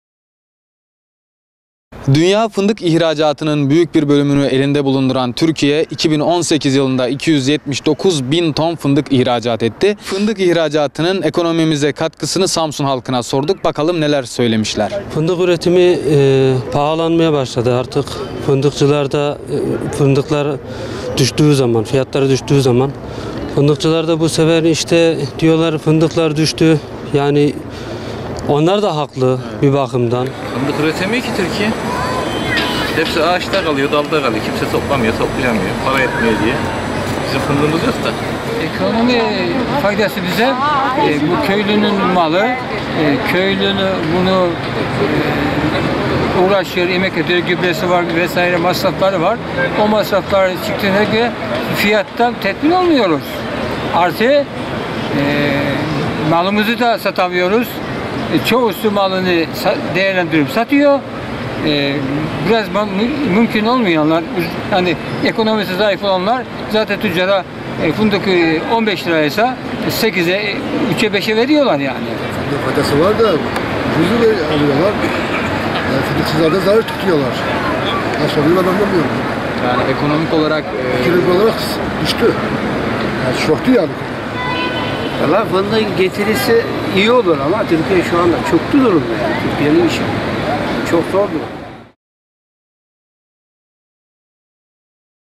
Samsun Konuşuyor: Fındık ihracatı geçen yıl 1,6 Milyar Dolar kazandırdı
Fındık ihracatında geçen yıl 1,6 milyar dolar kazanç elde edildi. Samsun halkına düşüncelerini sorduk.